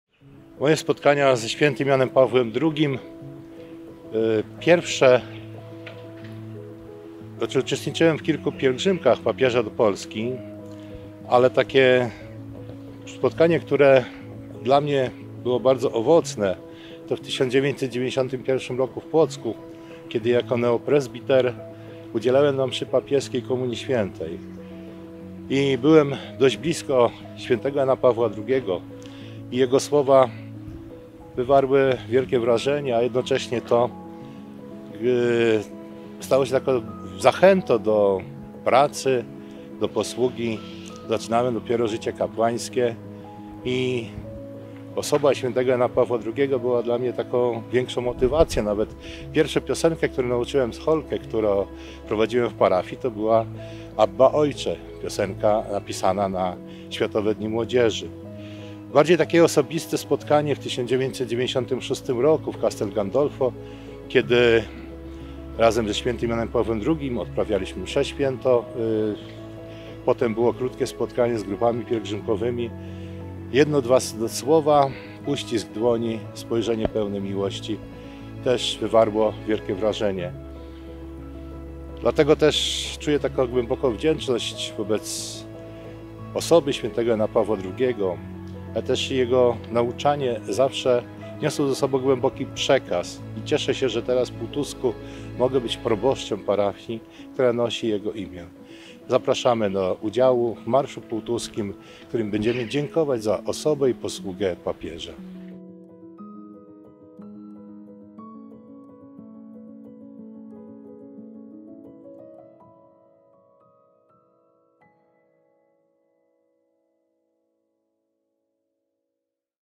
ks. kan. Sławomir Stefański o Janie Pawle II- Zaproszenie na Pułtuski Marsz Papieski
Społeczna grupa inicjatywna serdecznie zaprasza do udziału w Pułtuskim Marszu Papieskim, który odbędzie się w niedzielę, 2 kwietnia 2023 r.

Uroczystość rozpoczniemy o godz. 15:00 od uczestnictwa we Mszy Świętej dziękczynnej za pontyfikat św. Jana Pawła II, w kościele św. Piotra i Pawła (kościół szkolny).

Po Mszy Św. przejdziemy ulicami miasta (ul. Świętojańska, ul. Rynek) pod pomnik św. Jana Pawła II znajdujący się przy Bazylice.

Wszystkich, którym postać Papieża Polaka jest bliska zapraszamy do udziału w tej uroczystości.

Przyjdź i daj świadectwo.